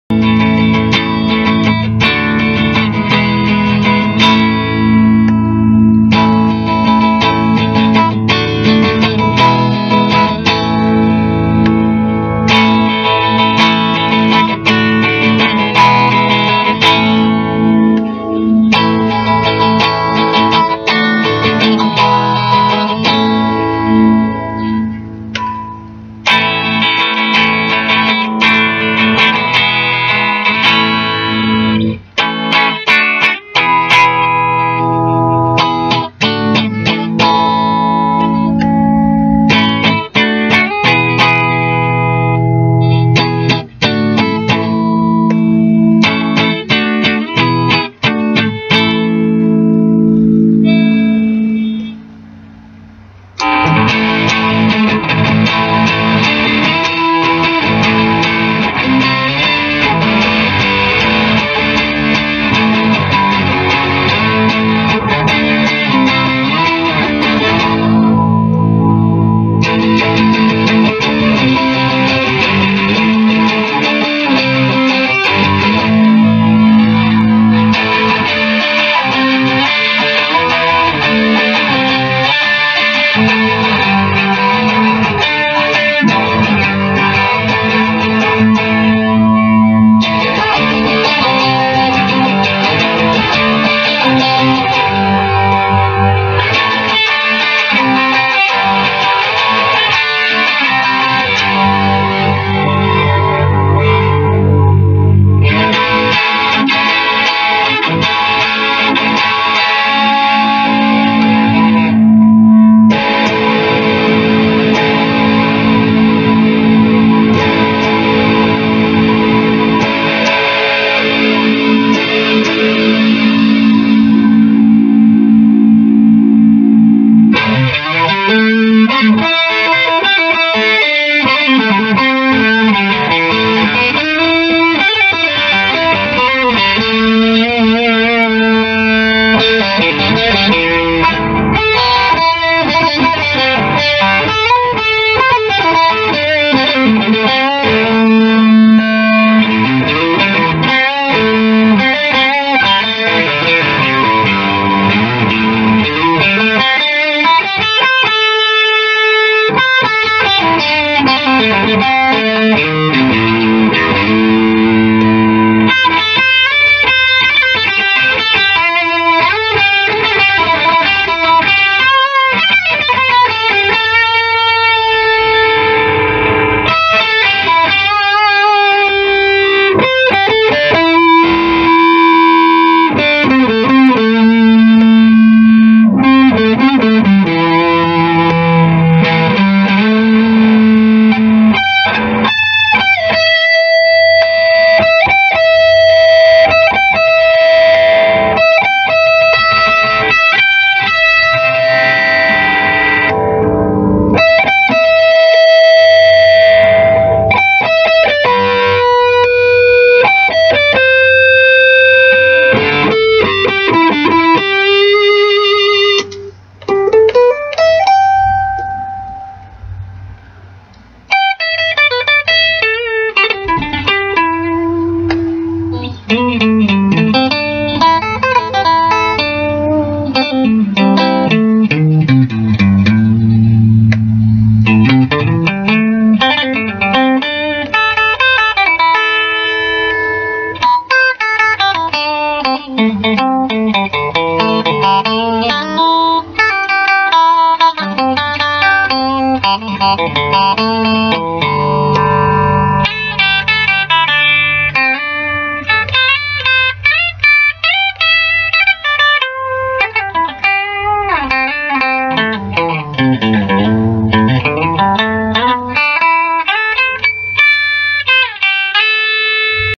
Stadium NY-111 Demo
This Stadium NY-111 is an unfamiliar name, but it doesn't mean it's not a player's guitar. It plays great, sounds great and the swirl paint job with nitrocellulose lacquer finish looks great! With a professional setup, it has excellent playability, very much like a Strat. DR Hi-Def Strings top it all off.